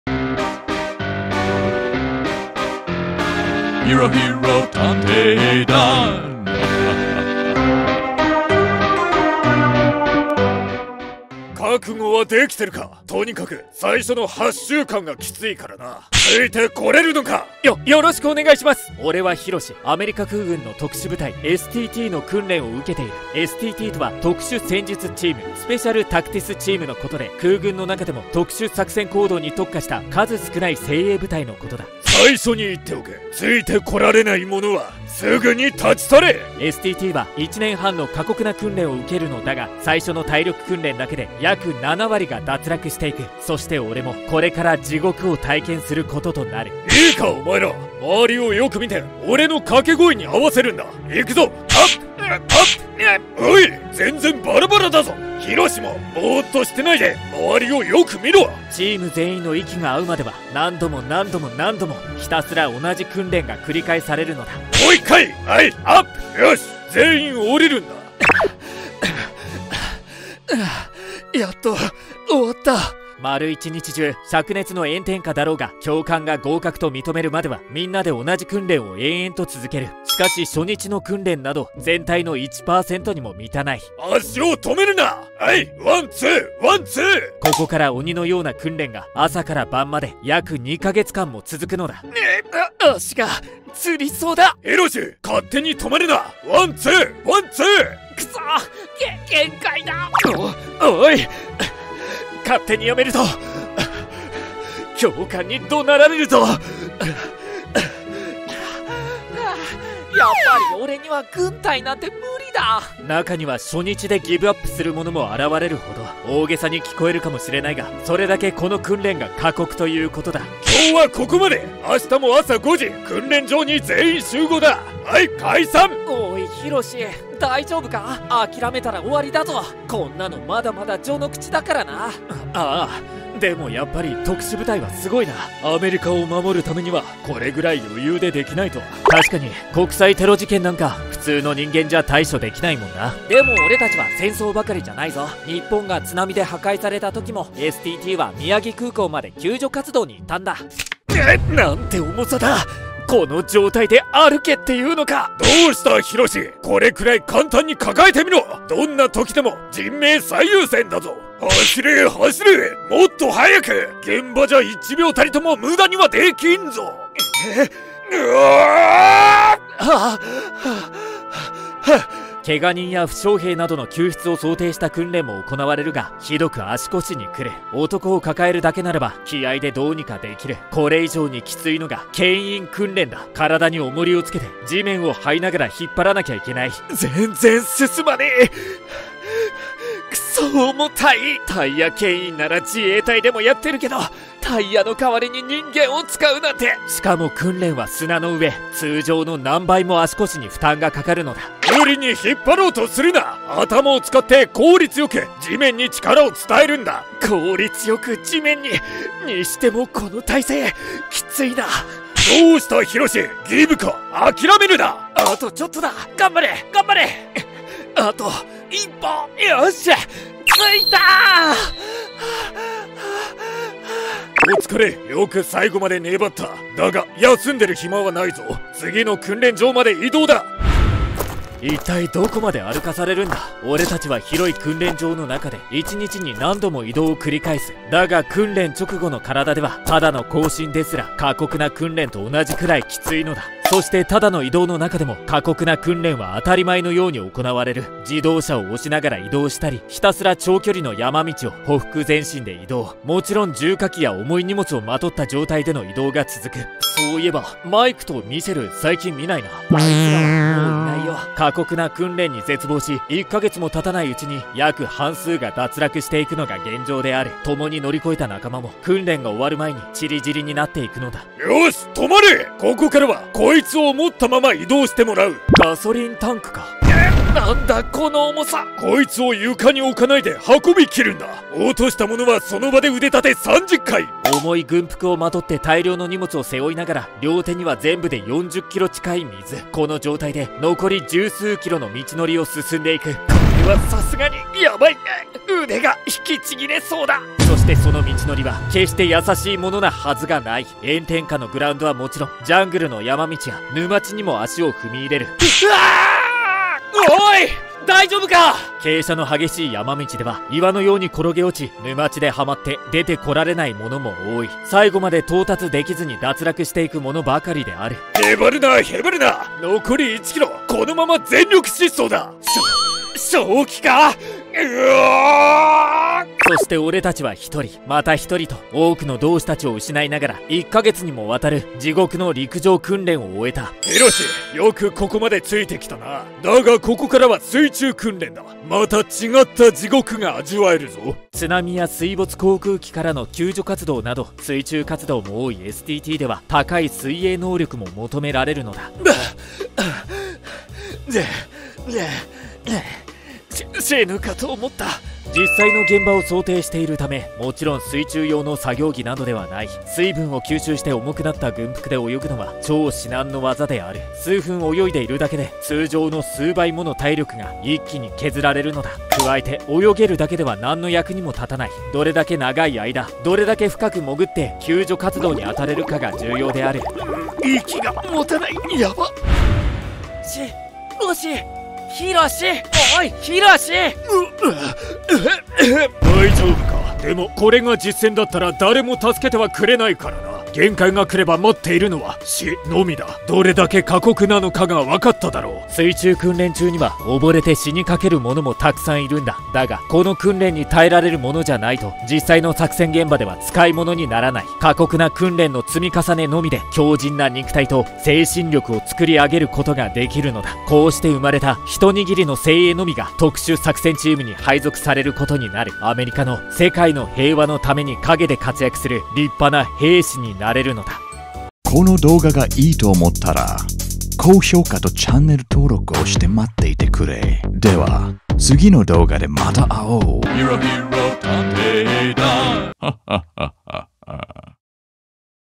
【漫画】７割が脱落するアメリカ空軍の特殊部隊（STT）のエゲツない訓練の実態（マンガ動画）
こういった方々に国は守られているのですね。

→通知をオンにすると新作公開時に通知が受け取れます！

・お仕事のご相談等、各種ご相談

こちらにご連絡いただければ改善・修正させていただきます。

◆クレジット
・BGM